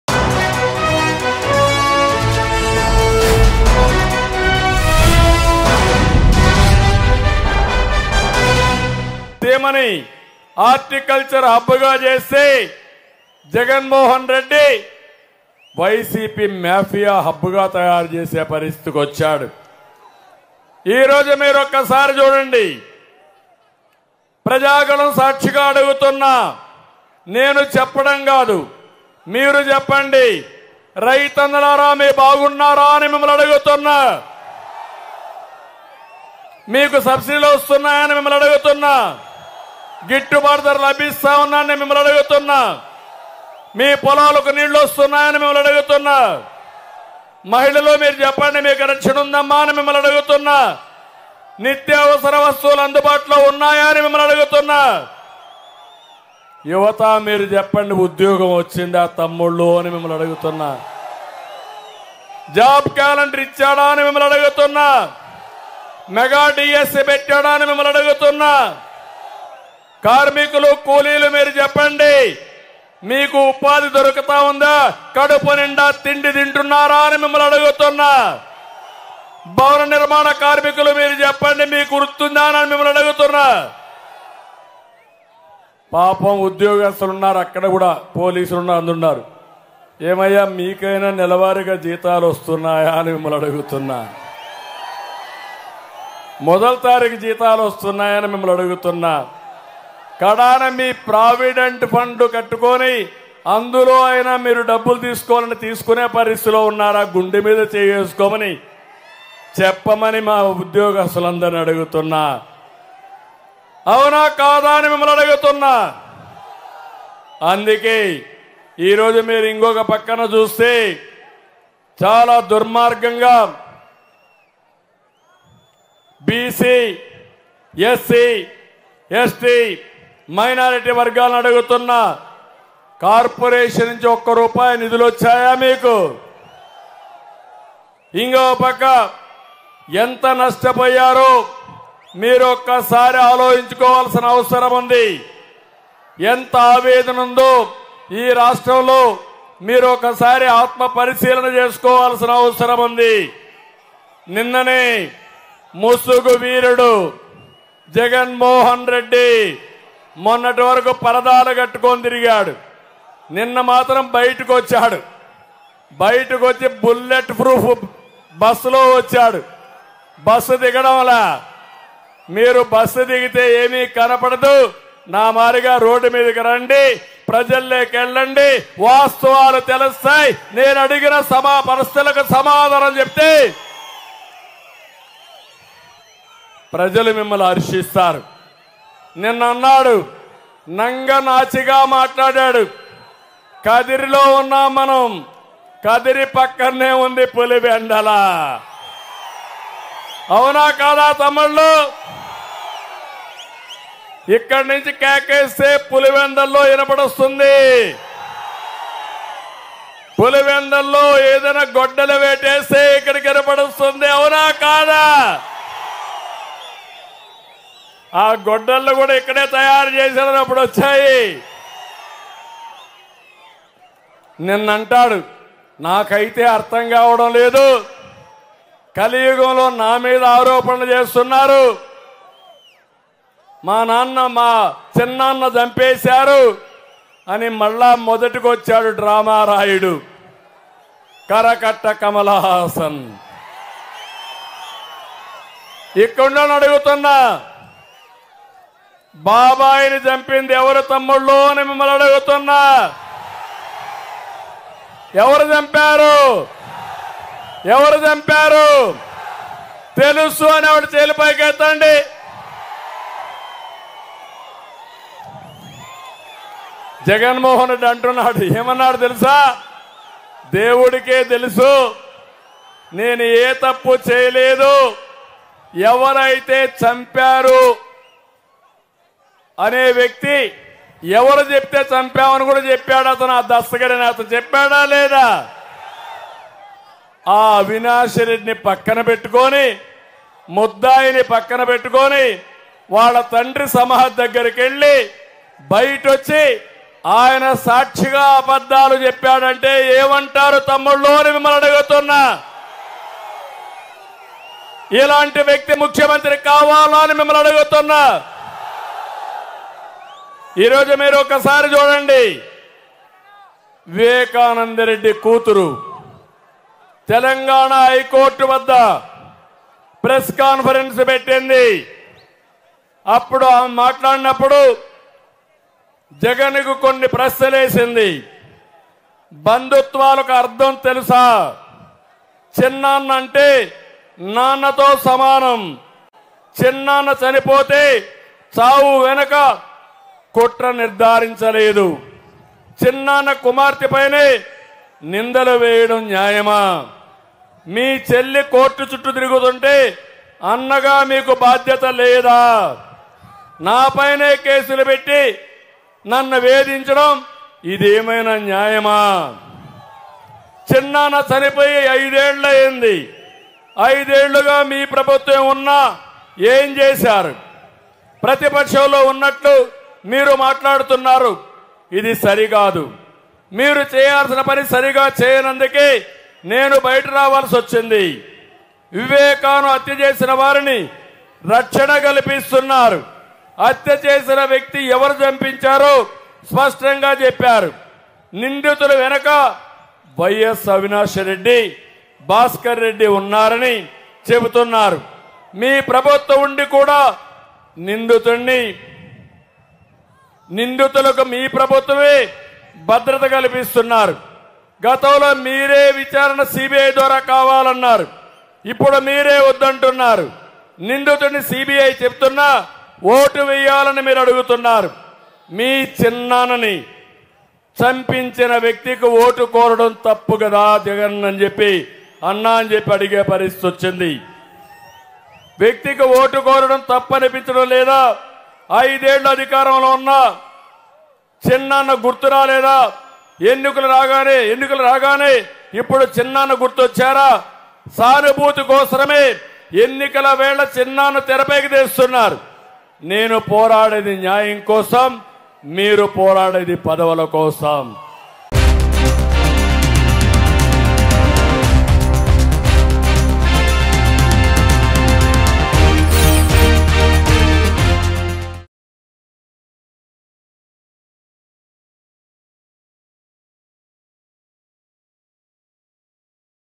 Chandrababu Naidu Sensational Comments On CM Jagan Bus Yatra | AP Elections 2024 | Mango News
Chandrababu Naidu Sensational Comments On CM Jagan Bus Yatra. He talked about CM Jagan's bus yatra.